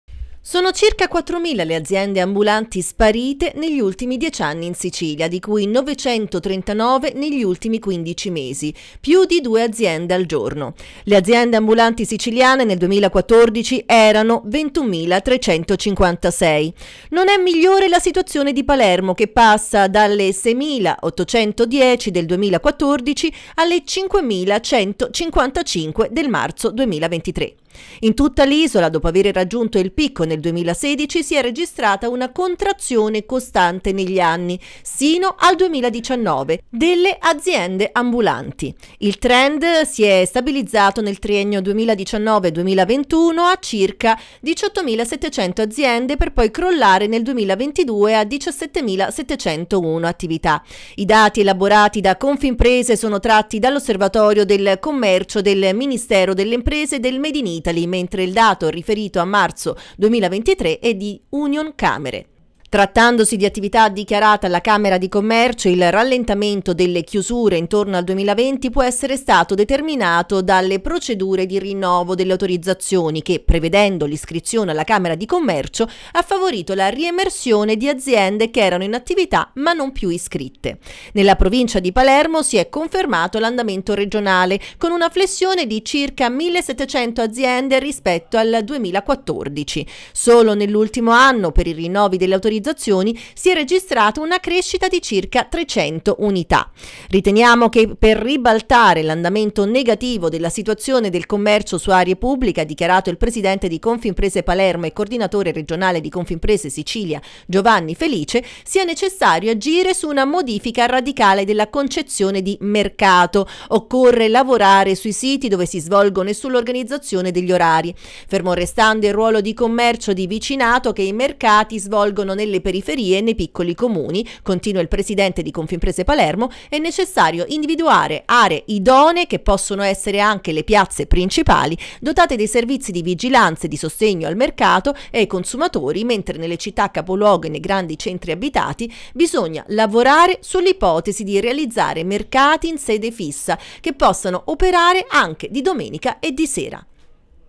Confimprese Sicilia: negli ultimi dieci anni 4000 aziende ambulanti in meno
Giovanni Felice presidente Confimprese Palermo dichiara : Occorrono profonde modifiche  gestionali, strutturali ed organizzative